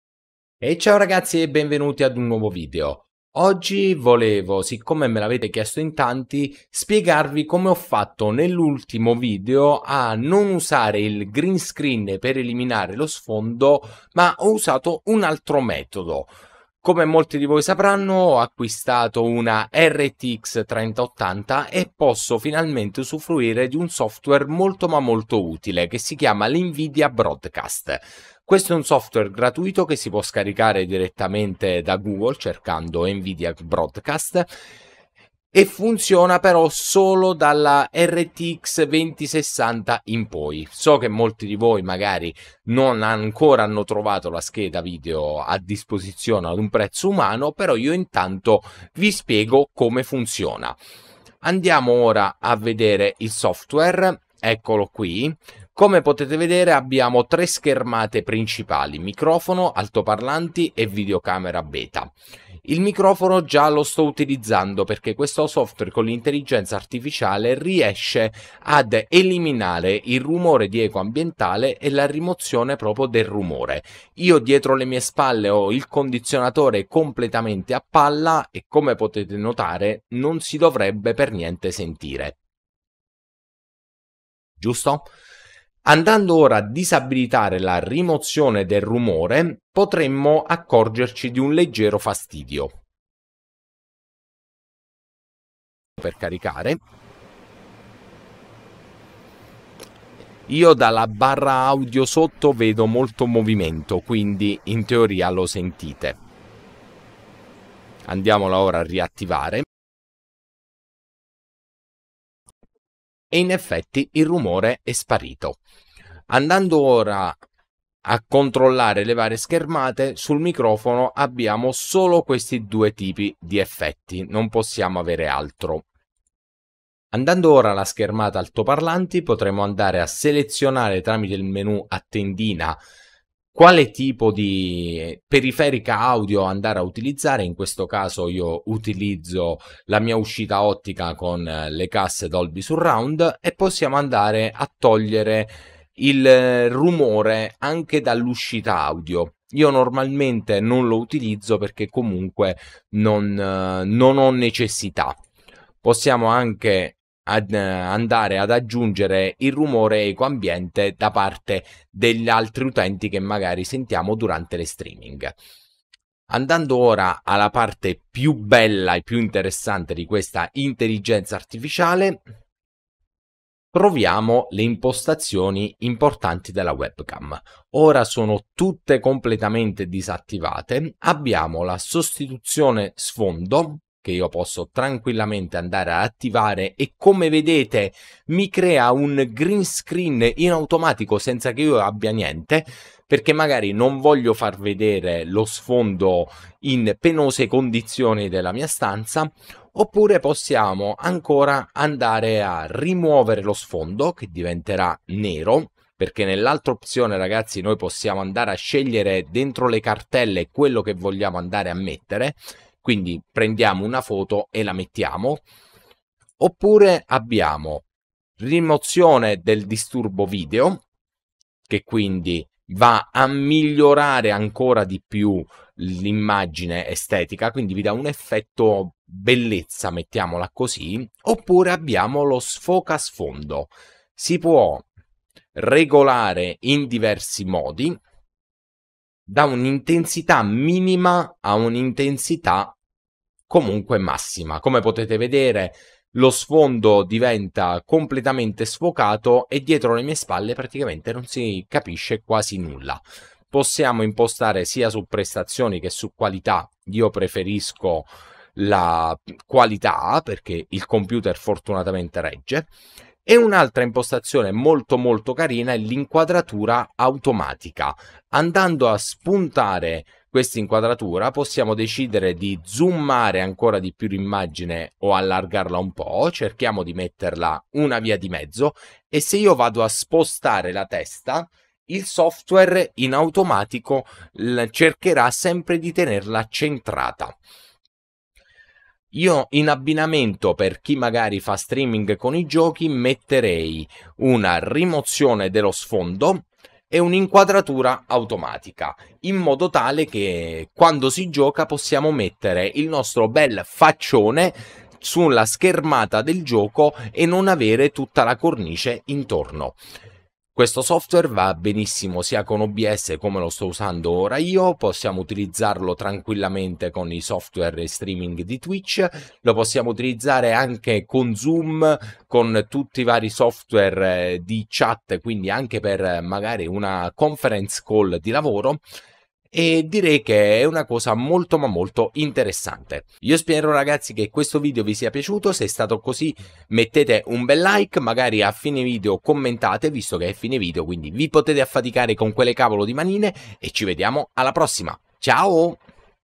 TEST Nvidia Broadcast - Come Funziona
Oggi vorrei farvi vedere un metodo molto semplice e pratico su come eliminare lo sfondo da una Live senza green screen!!!
Utilizzando proprio la nuova applicazione nvidia broadcast

COSA USO PER GIRARE:
GOPRO MAX
HERO 5 SESSION
HERO SESSION
TELECAMERA SONY
MICROFONO STUDIO
TESTA CAVALLETTO
LUCI STUDIO
GREEN SCREEN
BATTERY PACK